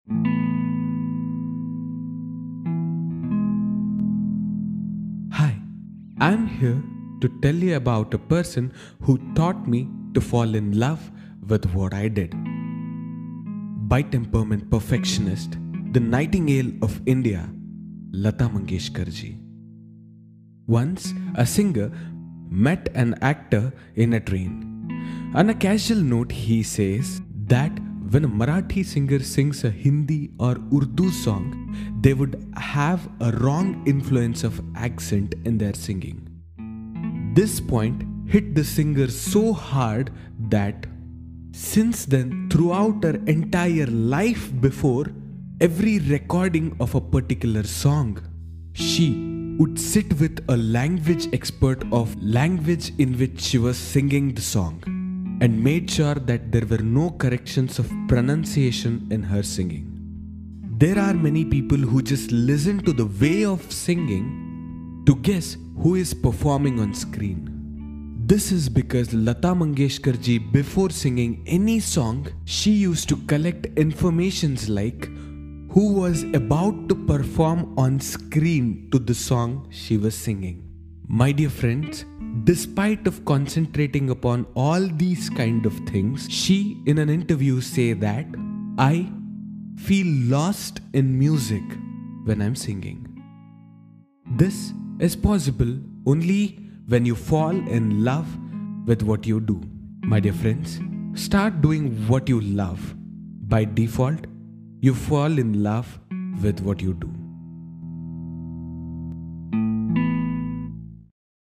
She taught me to fall in Love with what I do || Gunesh Bharathiya Speaks
Start doing what you love by default you fall in Love with what you do.

We Miss You Lata Mangeshkarji.

A Unit of Gunesh Bharathiya Foundation®